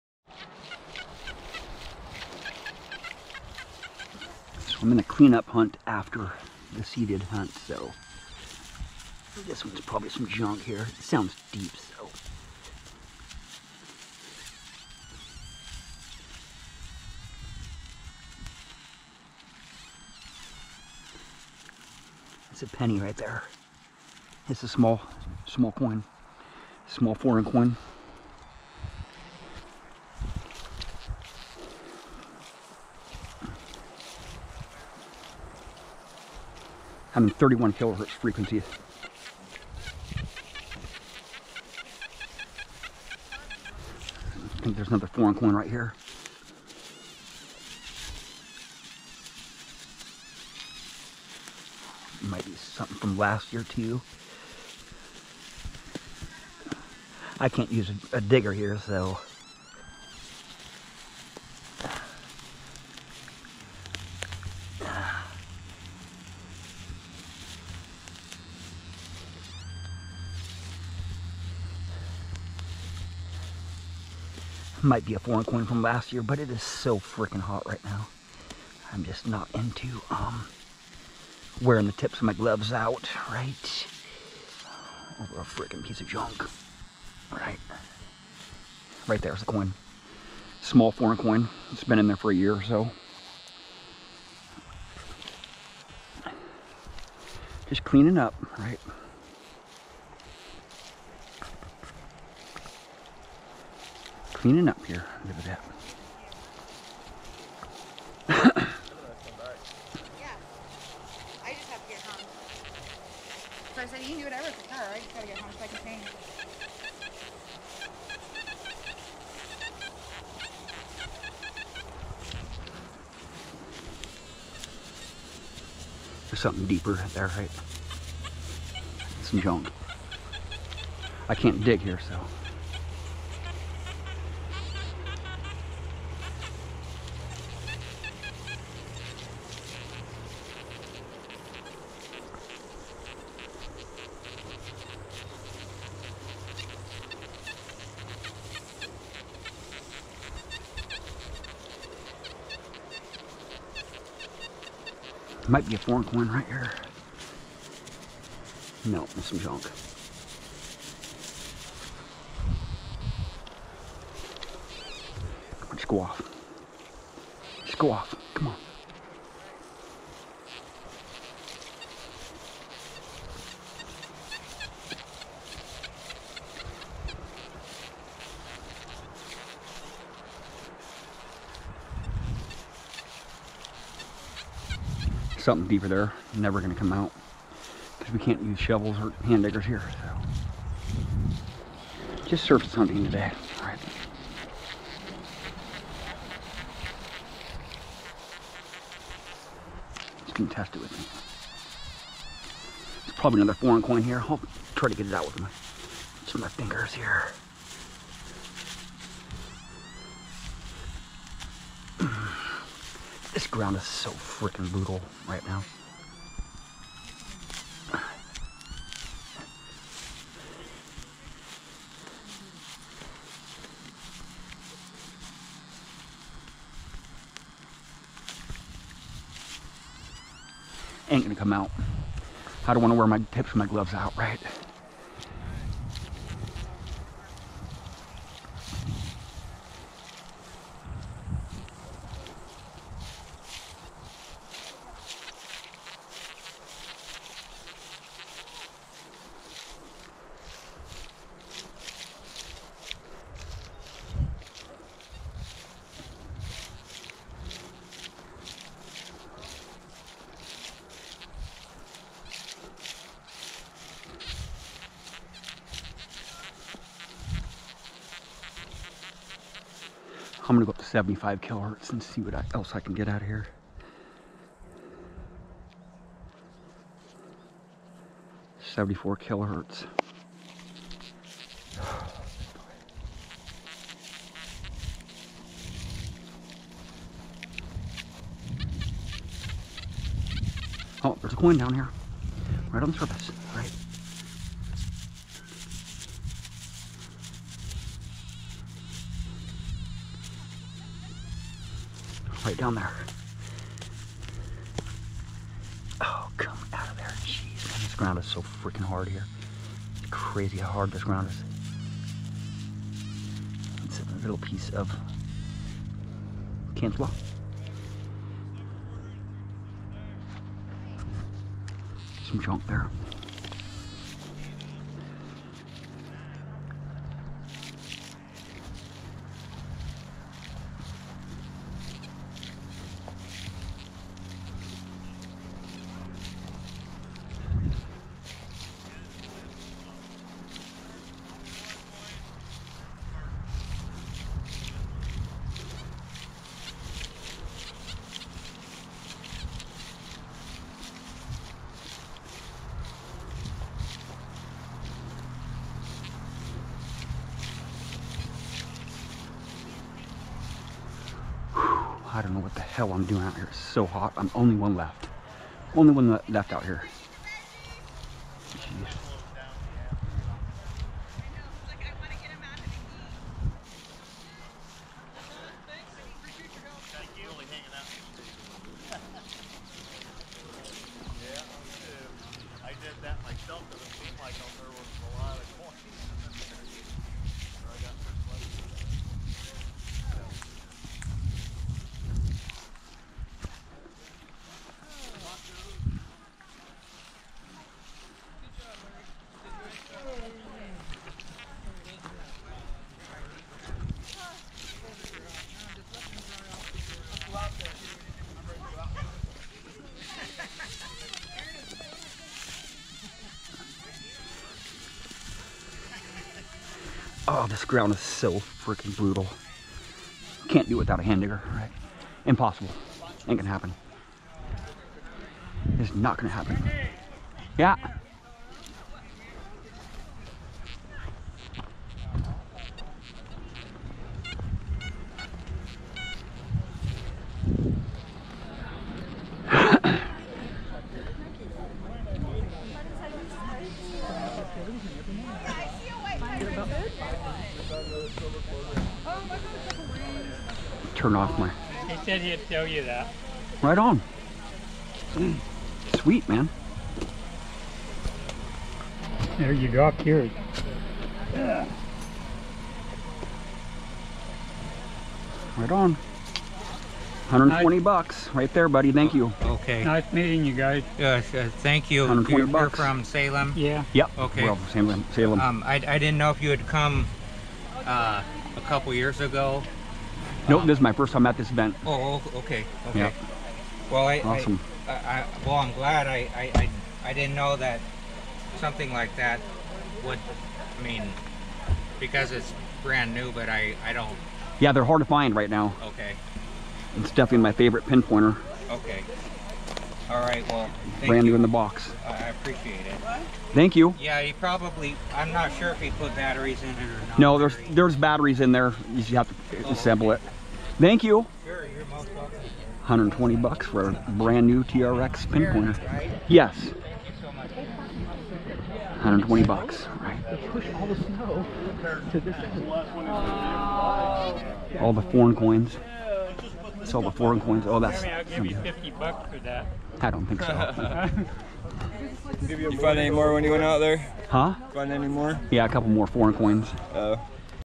I bought a unopened boxed Whites TRX pinpointer at our club hunt......
for $120 cash